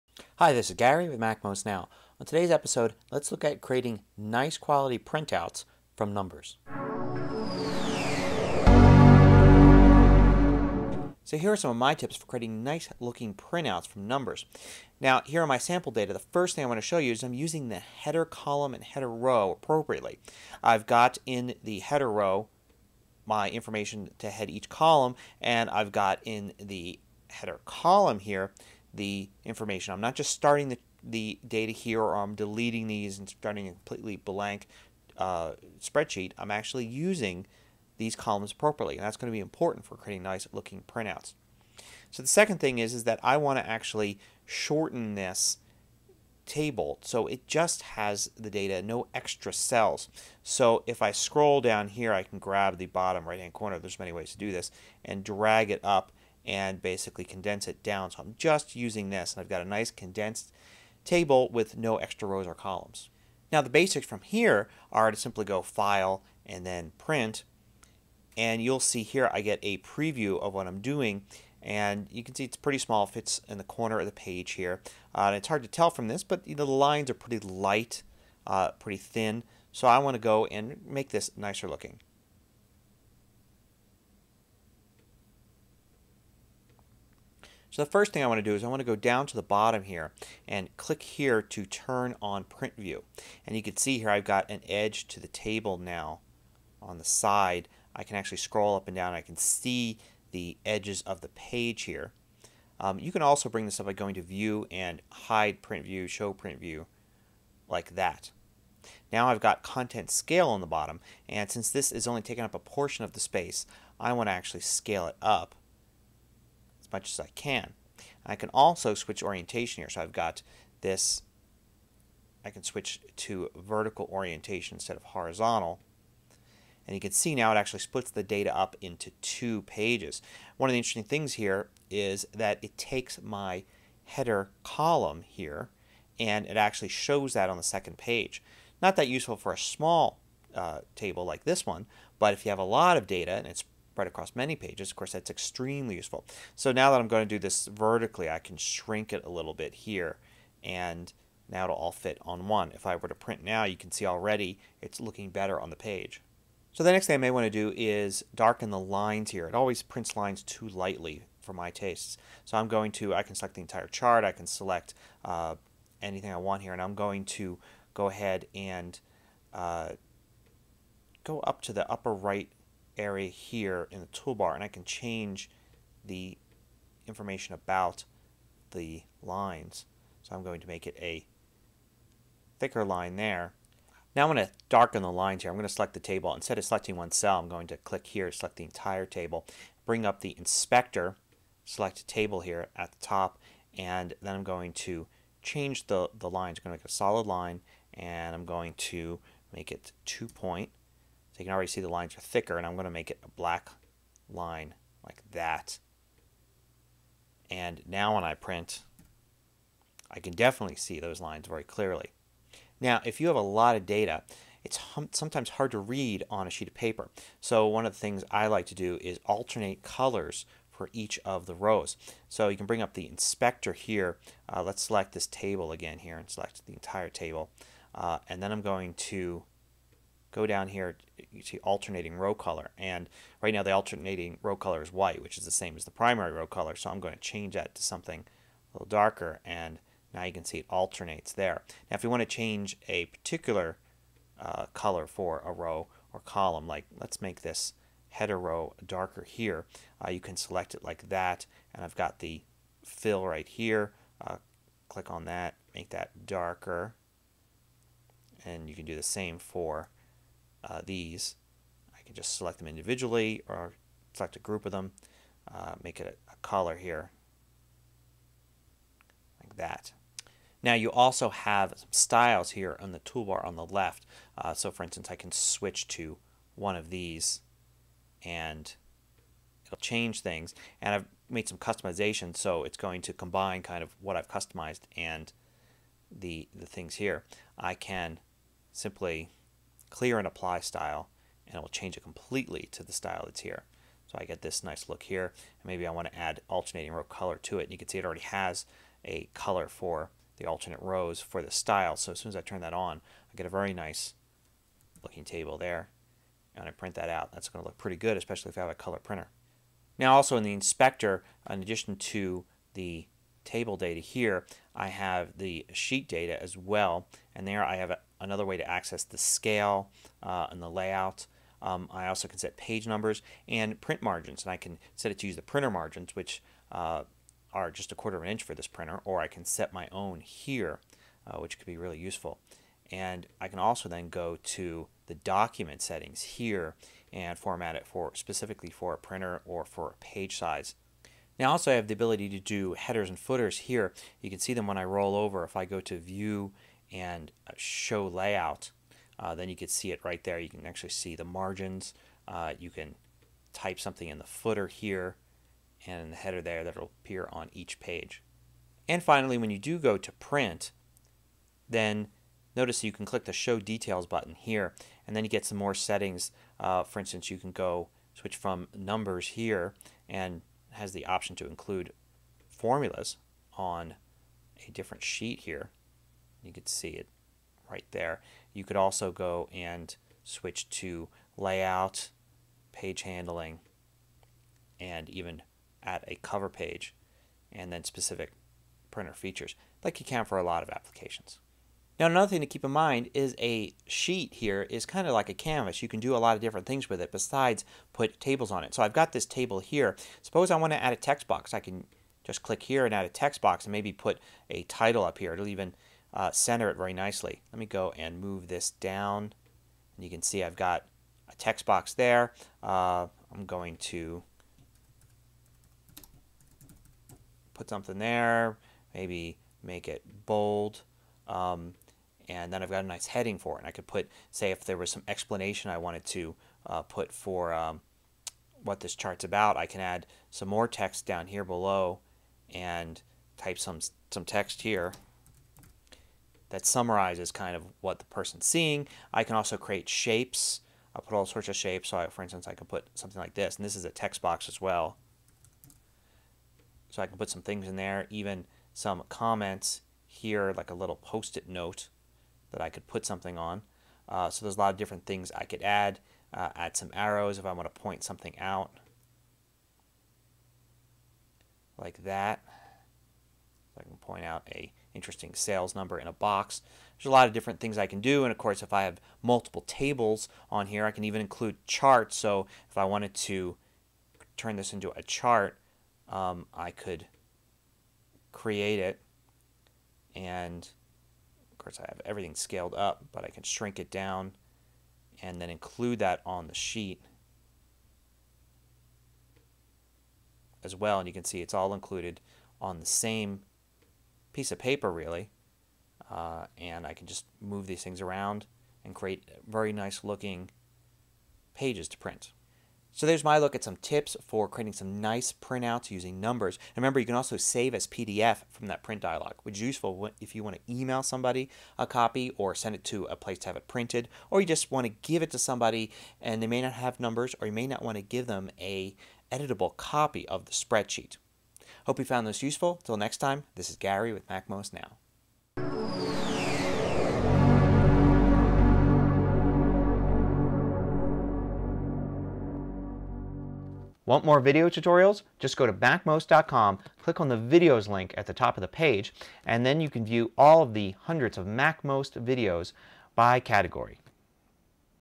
iWork Numbers Printing Tips (MacMost Now 649)
for comments and questions.) You can create nice-looking printouts from Numbers, but the options to do so aren't always obvious. Learn how to view your spreadsheets in layout view, adjust scaling, darken borders and use styles and design elements to create nicer Numbers printouts.